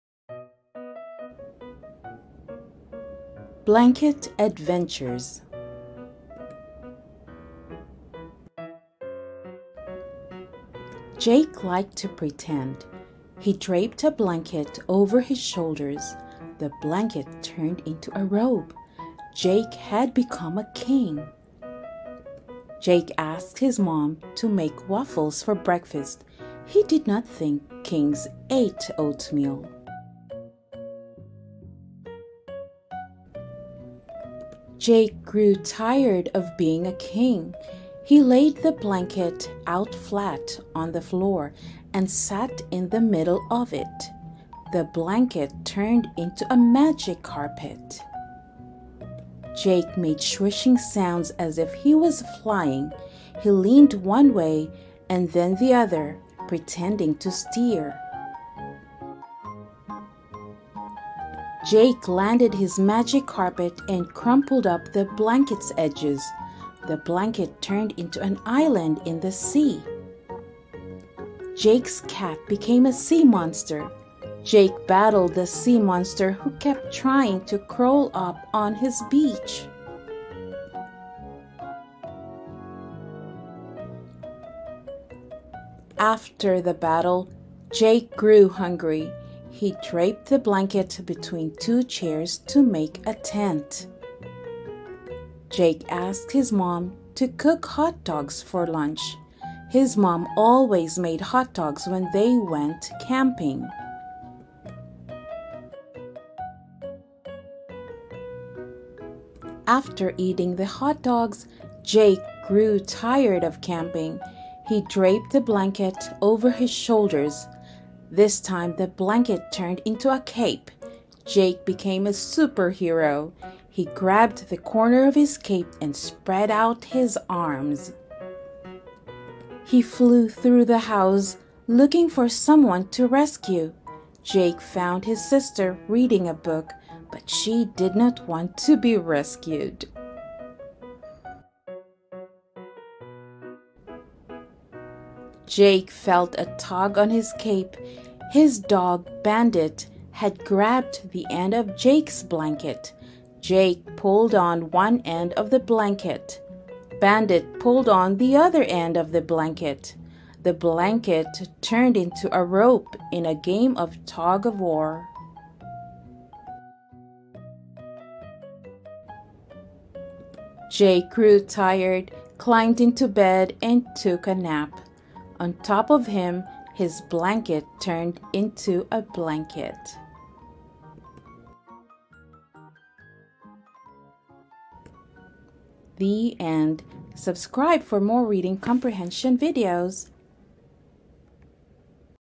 Blanket Adventures Story for Reading and Listening Comprehension
Blanket Adventures is a short story about a boy, his blanket, and his imagination